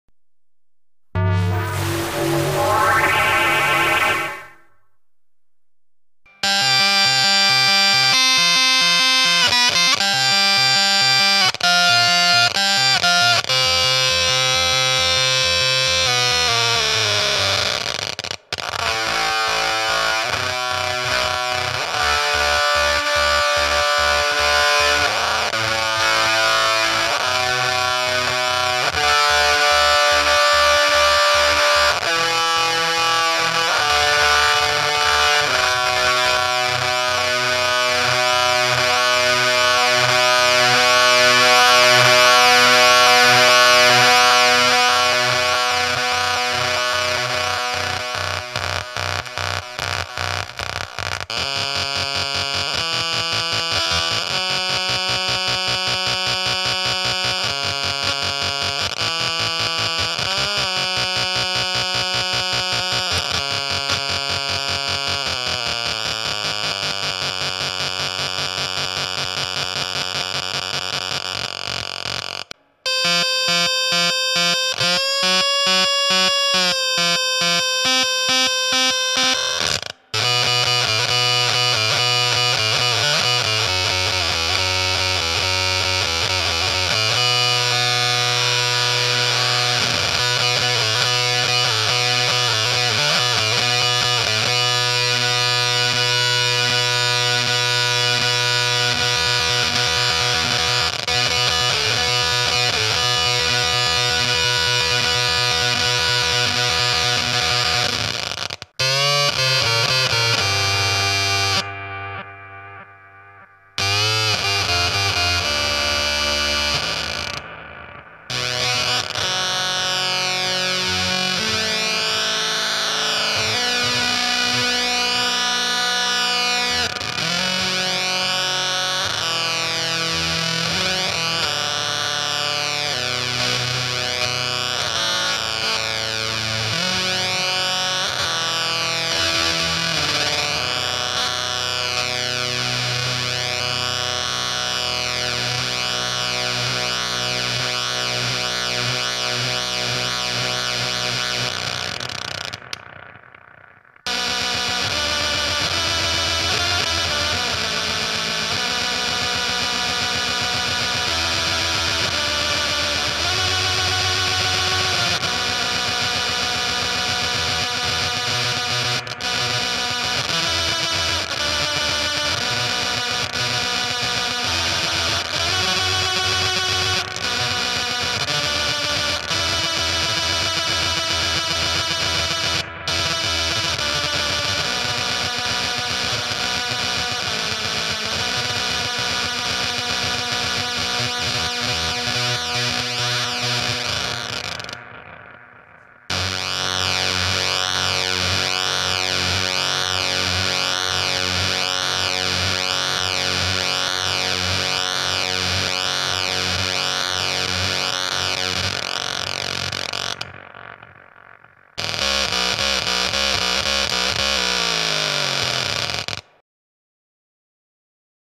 The Arcadiator by Kuato Design
The Arcadiator, a new video-game'ish fuzz.
It does glitchy square-wave fuzz, pulse width modulation (similar to Tim Escobedo's PWM), octave down and modulation that alternates beween two octaves.
The Bonus Stage introduces an extra LFO that takes over the Mass (Width) control and sweeps through the whole range up and down. It has a sound of it's own, something I would describe almost as a through-zero phasing!

CONTROLS
PWM switch - this toggles between 'pwm' or 'octave up'
WIDTH pot - this controls either the width of the square wave (pwm) or the intensity of the octave up

BLEND pot - mixes between 'either PWM or Octave up' and the 'Octave Down'

OCTAVE switch - This toggles between one or two octaves down
MODE switch - this turns the Octave down on or off (except when the LFO is on)

MODULATION
LFO switch - turns on/off the modulation that is alternating between one or two octaves down, or:
MODE switch - Toggles between either one or two octaves down or just one octave down and unison or a choppy tremolo of the octave down (depending on how the mix is set)